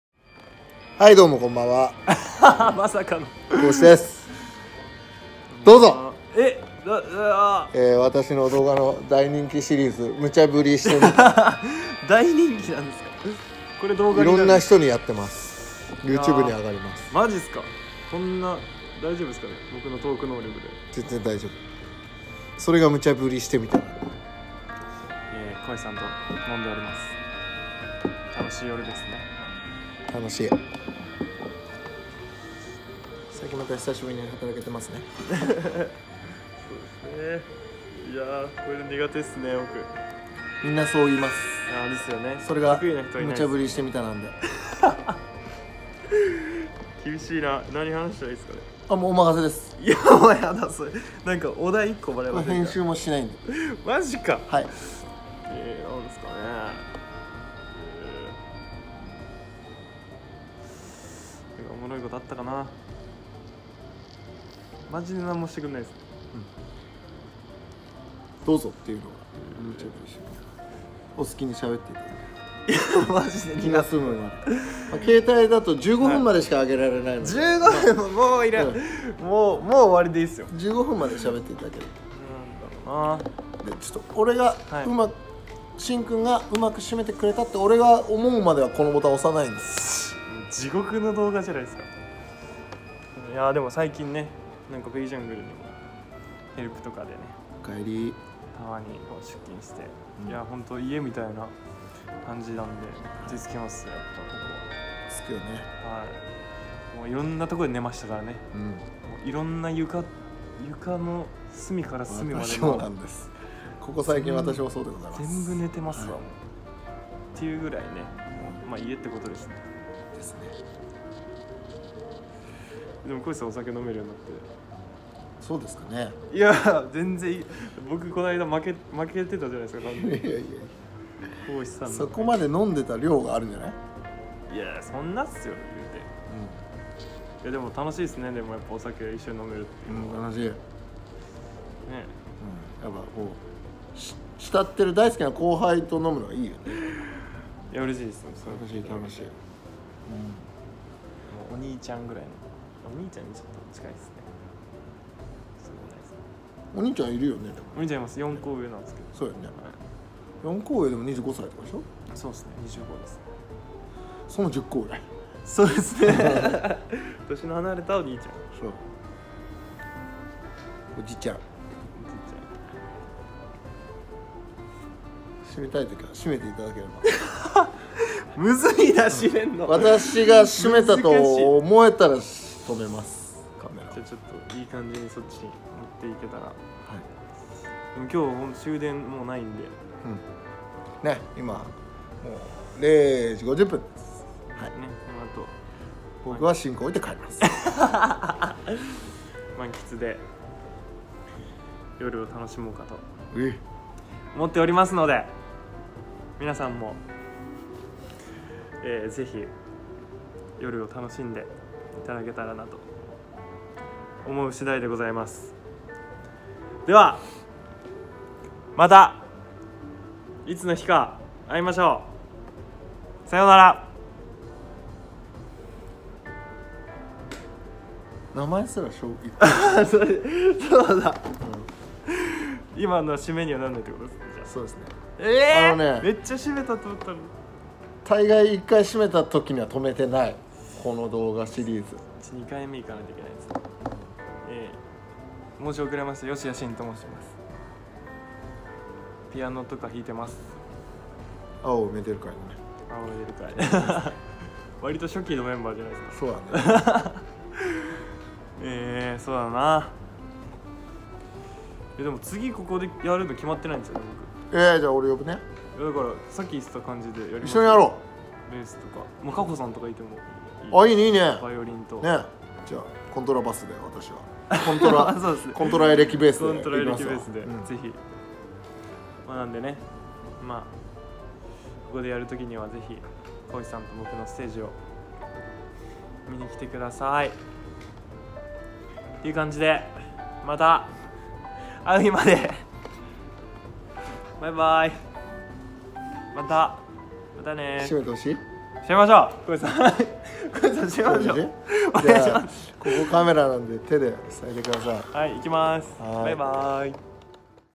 【イケメンとまったりトーク】彼女は夢を見る、吉屋真くんに無茶振りしてみた！！
今日はしんくんです！大好きな後輩です！！

動画#102